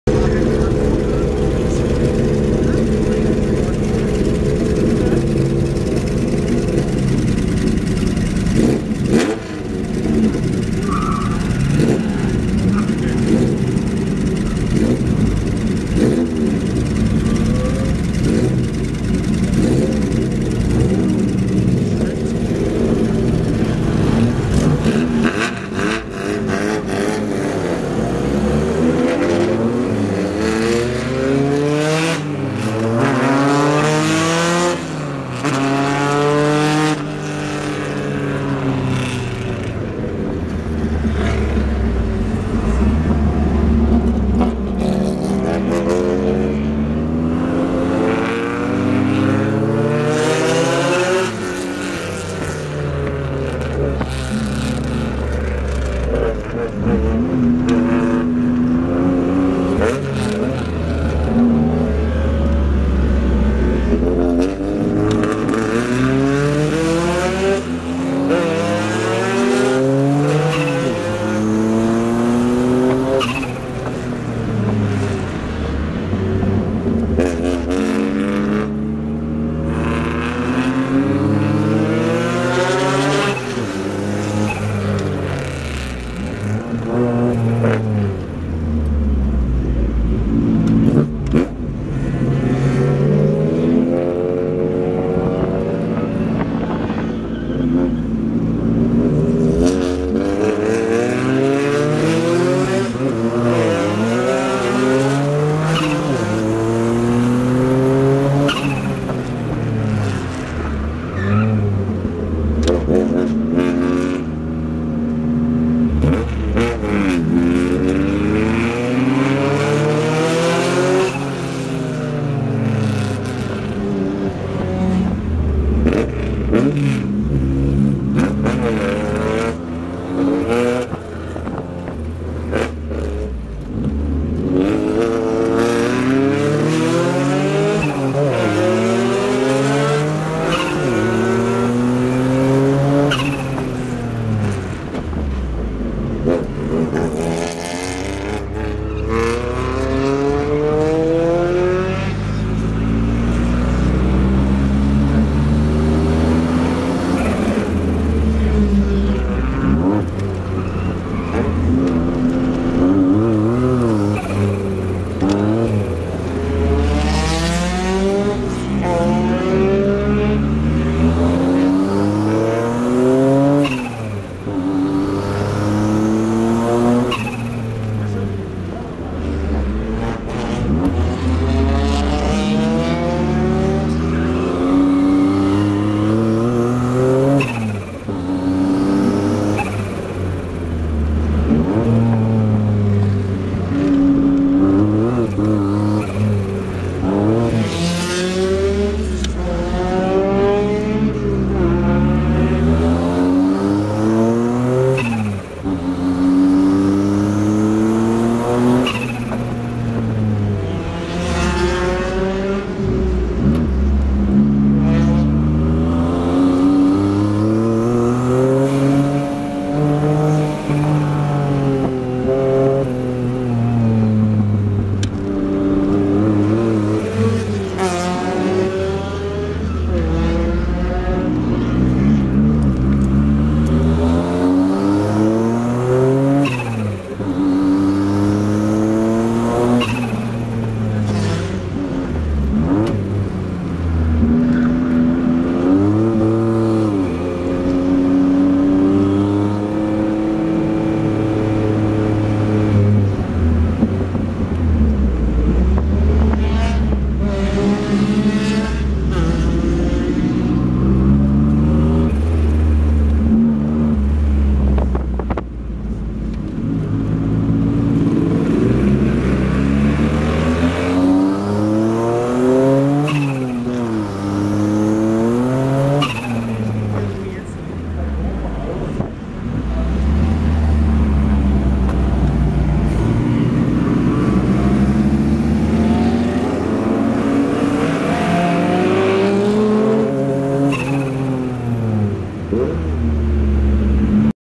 Onboard im Capri Turbo Klassikwelt Bodensee 2014, 1. Lauf der Gruppe 6 (Rennfahrzeuge)
Onboard im Ford Capri Turbo auf der Klassikwelt Bodensee, 1. Lauf am Freitag in der Gruppe 6 (Rennfahrzeuge).
Wenn man genau hinschaut, sieht und hört man das ab etwa 2:20 die Kuplung nicht mehr funktioniert, die nächsten Runden musste dann ohne Kupplung geschalten werden, was einen nicht gerade schneller macht ;)

Mit dabei Crosslé 9S,  Porsche 911, Porsche 928, BMW 2002 Alpina Gruppe 2, Ford Capri 3.0 Essex, Ford Capri 2.8i Turbo, Mercedes SL R107, Shelby Daytona Coupe, Lotus Europa